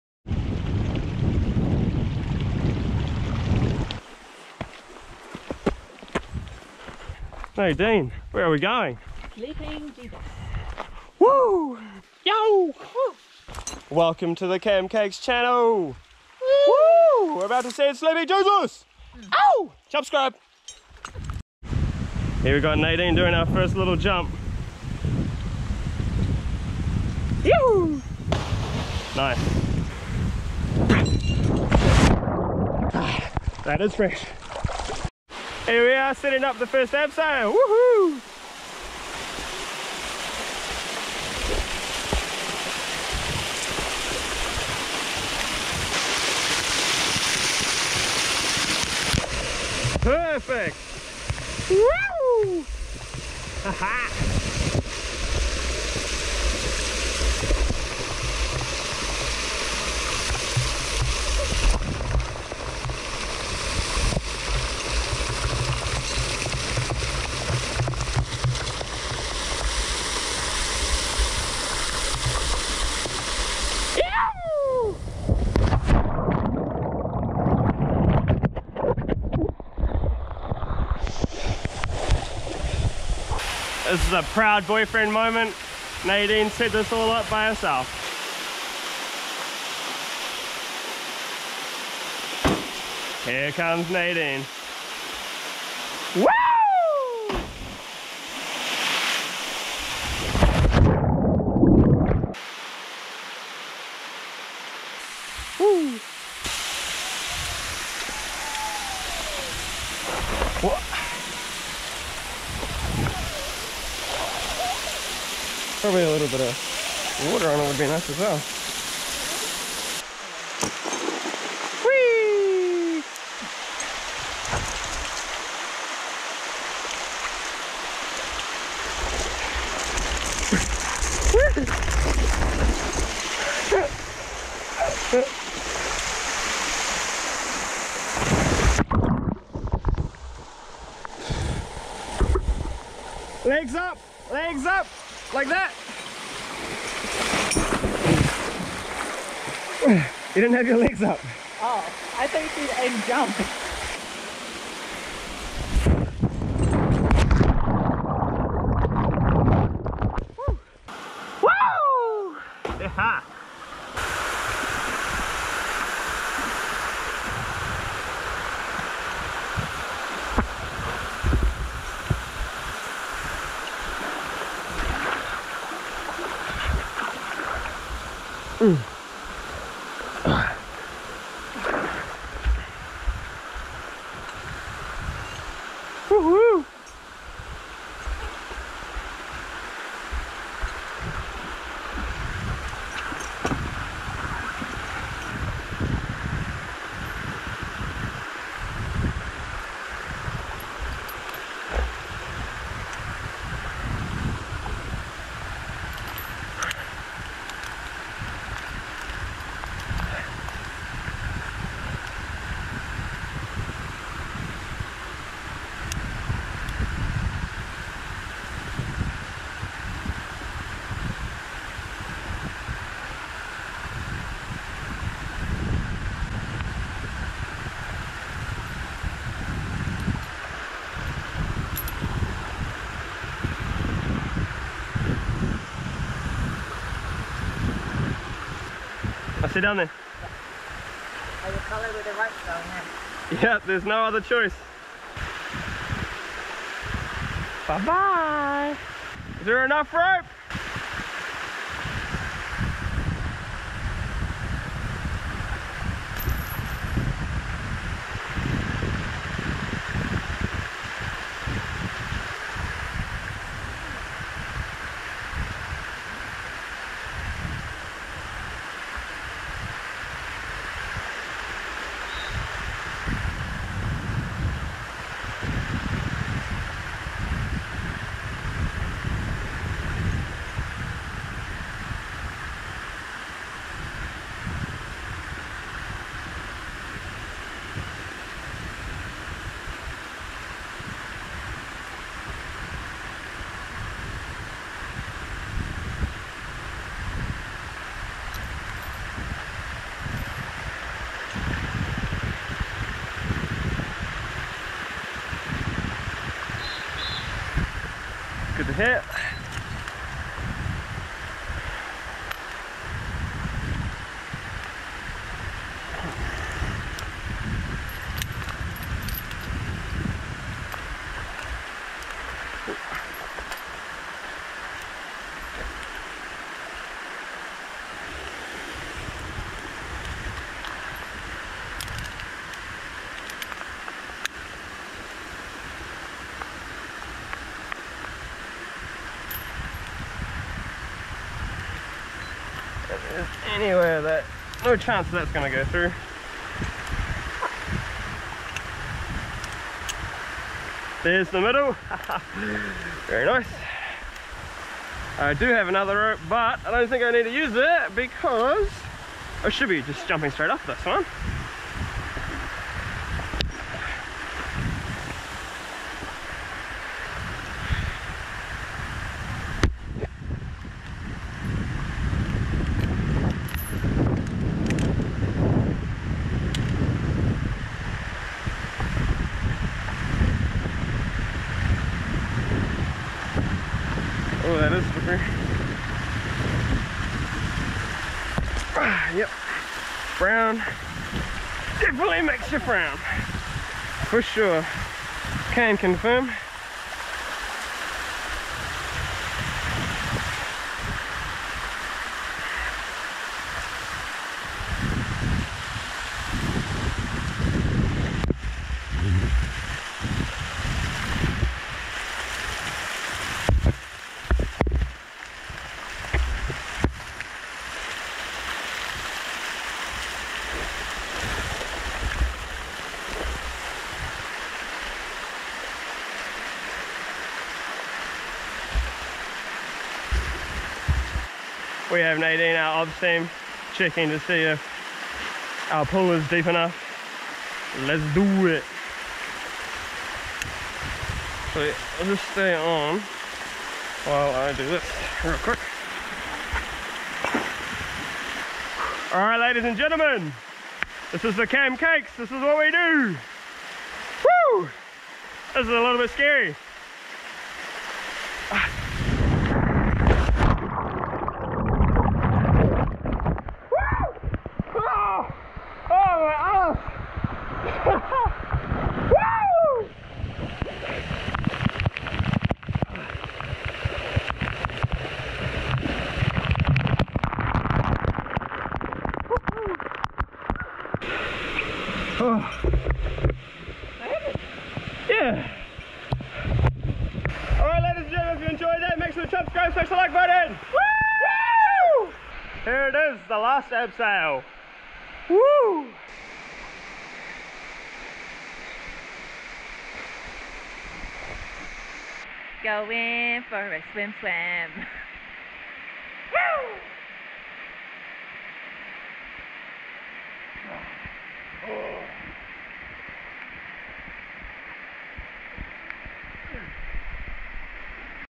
GIRL GOES THROUGH CANYON | SLEEPING GOD
Thats right, this hot af lass is jumping off cliffs like a pro ;)